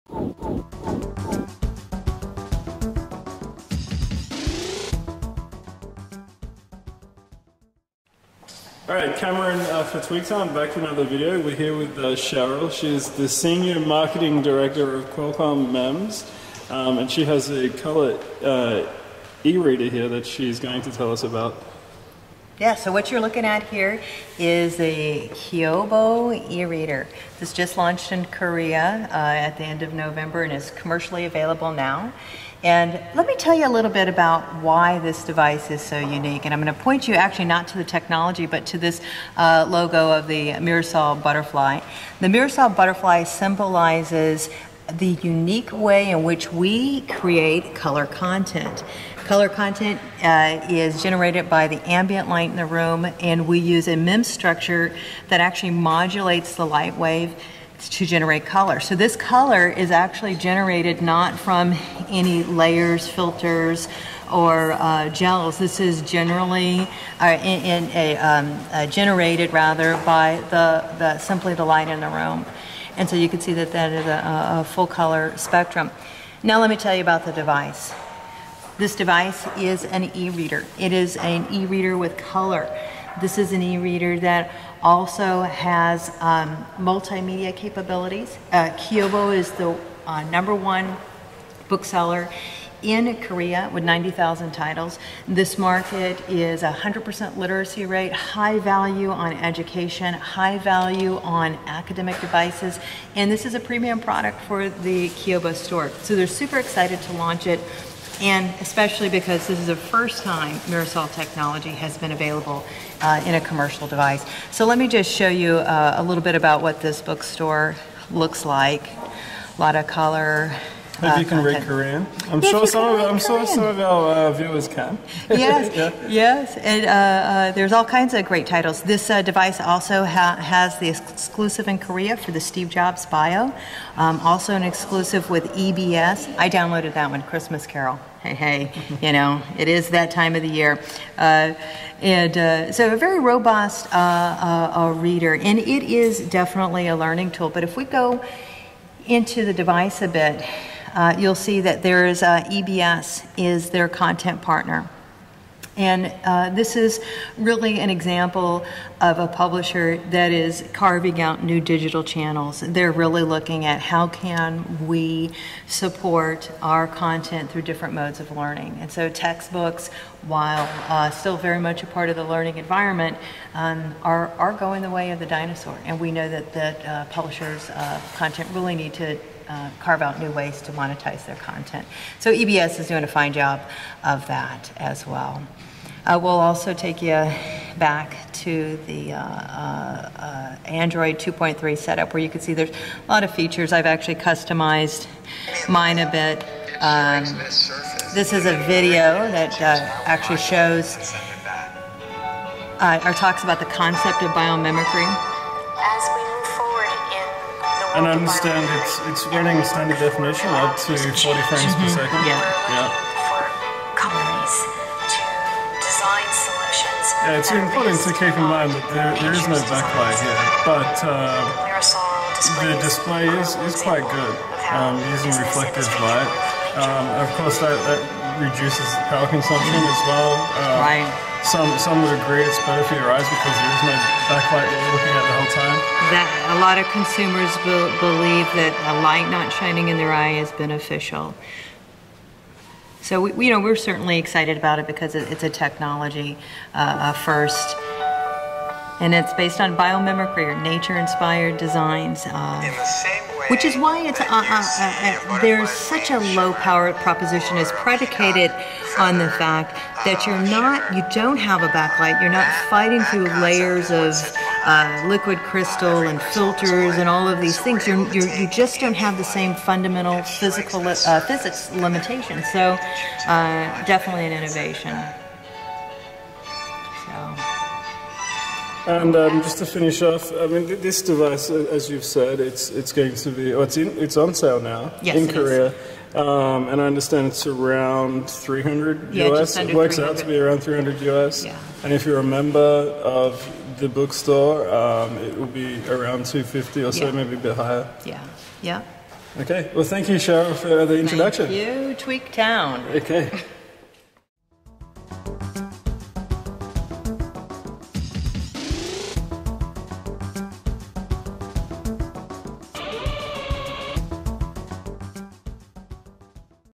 Hands-on with Kyobo color e-reader using Qualcomm's mirasol display
Earlier on today we attended a Qualcomm media roundtable in Taipei where we were introduced to a partner product of theirs from a Korean bookstore company called Kyobo.

Kyobo is famous in Korea and its digital catalog includes some 90,000 titles including regular ebooks, children's books, video books and video lectures. With almost a 100% literacy rate and an aggressive government very keen on high levels of education and parents with enough deposable income for youth learning gadgets, Korea is a perfect market for Qualcomm to spread its wings and work with its partners to bundle its mirasol display technology on mobile products.

The result of said above is the launch of the first color e-reader featuring Qualcomm's mirasol display tech. Mirasol is different to that of LCD, LED or plasma in that there are no harmful chemicals used in the production of the display and there is no back light, which some consider harmful to human eyes over extended periods of use. Mirasol reflects natural ambient light in your surrounding and uses it to create the color display. So, you aren't going to get the same quality display as LCD, but Qualcomm are not trying to compete with LCD and mirasol displays use less power. This is only going to result in a device with longer battery life at the end of the day.

The Kyobo e-reader features a 5.7-inch display with a 1024 x 768 resolution and is powered by Qualcomm's 1GHz Snapdragon S2 processor. By default it ships with Android 2.3 and a select few Android apps. It is able to provide basic web surfing with built-in Wi-Fi and it supports standard definition video playback up to 40 frames per second. As mentioned by Qualcomm, it's important to remember this device is not a tablet and was never designed to be. It is a device designed for students which offers adequate educational interactivity in my opinion.

It is already on sale now in Korea for $349,000 KRW ($310 USD) and if you are a member of the Kyobo Platinum Book Club it can be picked up for 299,000 ...